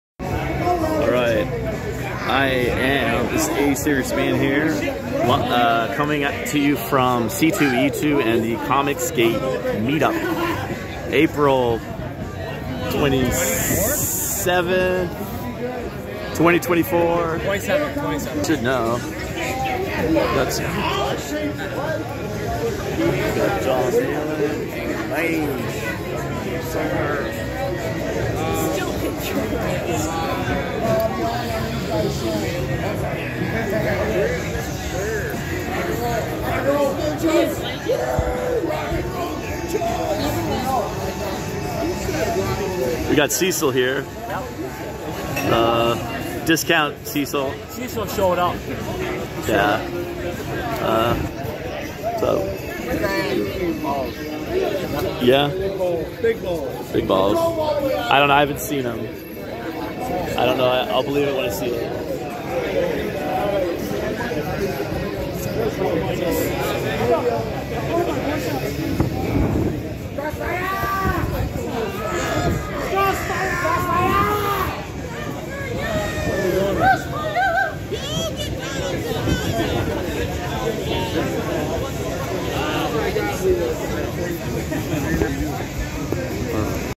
Discount Cecil and Comicsgate Meetup at c2e2
At the comicsgate meetup in Chicago with Jon Malin and Shane "Mange" Davis